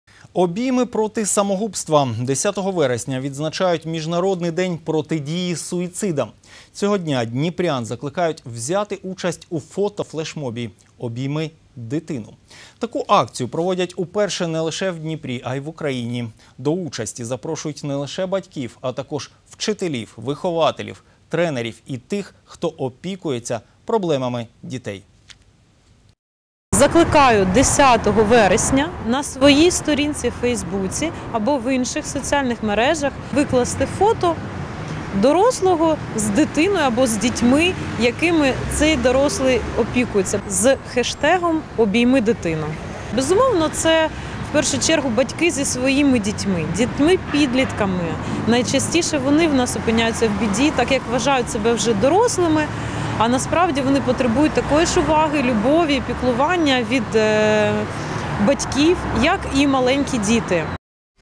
Обійми проти суїциду: 10 вересня міжнародний день протидії самогубствам
Десятого вересня відзначають міжнародний день протидії суїцидам. Цього дня дніпрян закликають взяти участь у фотофлешмобі “Обійми дитину”.  Таку акцію проводять уперше не лише в Дніпрі, а й в Україні. До участі  запрошують не лише батьків, а також вчителів, вихователів, тренерів та тих, хто опікується проблемами дітей.

СХ_ГАННА КАРПЕЧЕНКОВА НАЧАЛЬНИЦЯ ВІДДІЛУ ЮВЕНАЛЬНОЇ ПРЕВЕНЦІЇ ГУНП В ДНІПРОПЕТРОВСЬКІЙ ОБЛАСТІ
Закликаю 10 вересня на своїй сторінці у фейсбуці або інших соціальних мережах викласти фото дорослого з дитиною або з дітьми, якими ці дорослі опікуються з хештегом Обійми дитину. Безумовно — це батьки зі своїми дітьми, дітьми-підлітками. Найчастіше вони вже опиняються в біді, бо вважають себе дорослими. А насправді вони ж потребують такої уваги, любові від батьків як імаленькі діти.